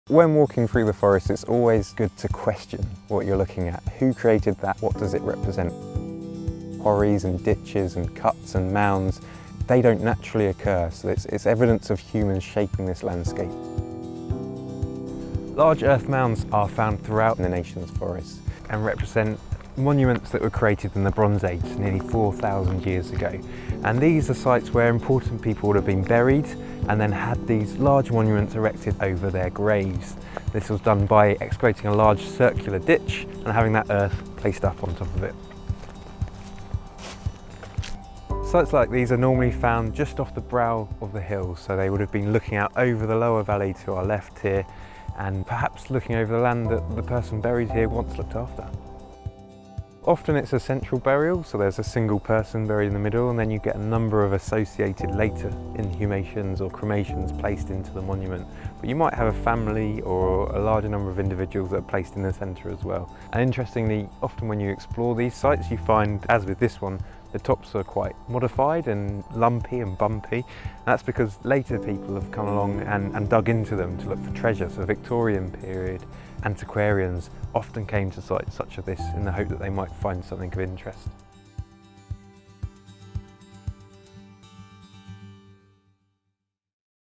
How to spot earthworks in the nation's forests | Landscapes of the past
Have you ever stopped to ask how your local forest came to be and why it looks like it does? With a closer look you'll likely spot earthworks and other features that tell stories of historical landscapes.

We invite you to start noticing evidence of past human activities that can tell a story of how people have interacted with these landscapes over thousands of years. You will see funny shaped lumps and bumps, arrow straight banks, feature trees or a concreate pad – all evidence of human activity. So how did these non-naturally occurring features get there and what were they for?